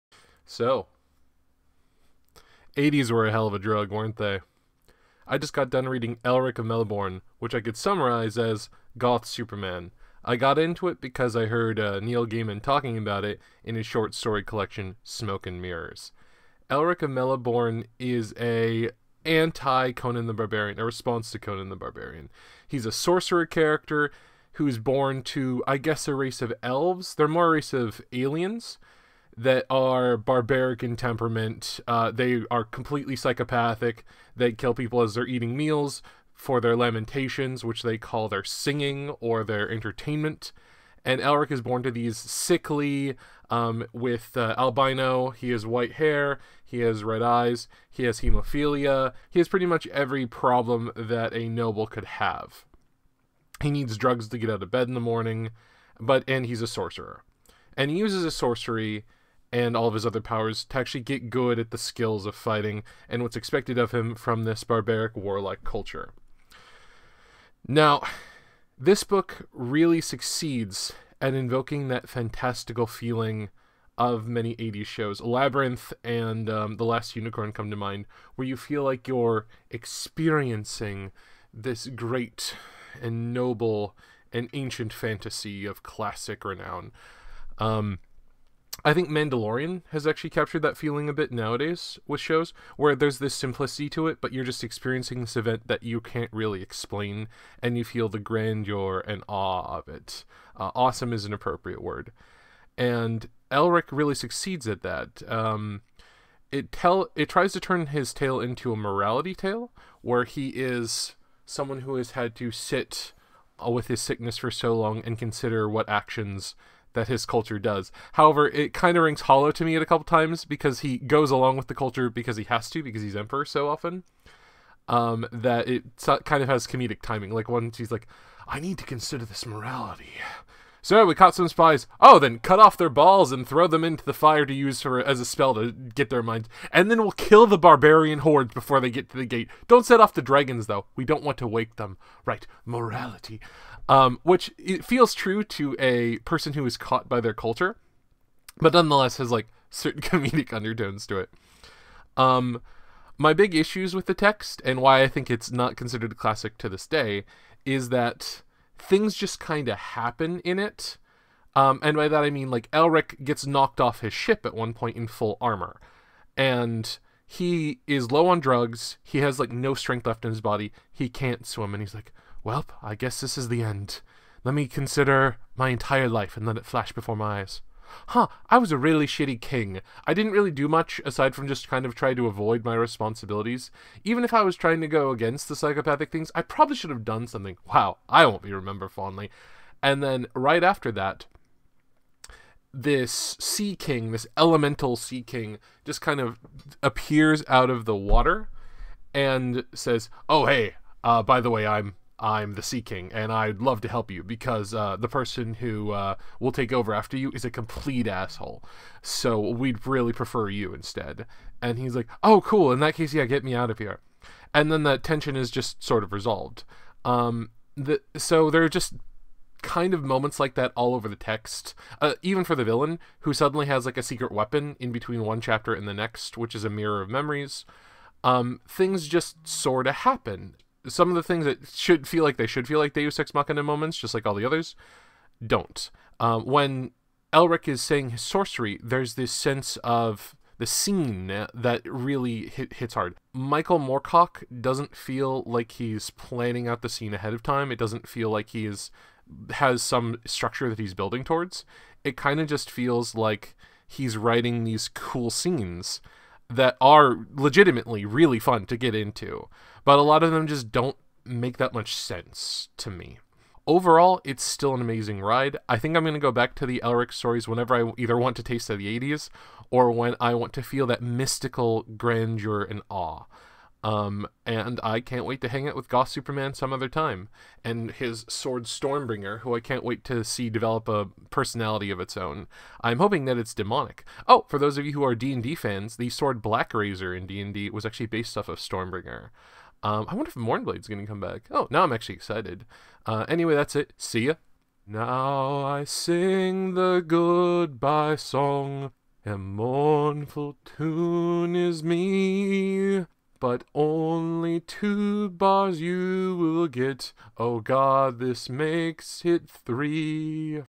ELRIC SAGA REVIEW 1📖 Elric of Melniboné
Elric is the 'moral' albino Conan that we all need in our lives.
The original white wolf is the emperor we need.
With pages free of bureaucracy, and a distance far away from us.

✏️Written by Michael Moorcock
🎨Book Cover Illustrated by Robert Gould

For your goth superman needs.

📚BOOKS MENTIONED📚

Elric of Melniboné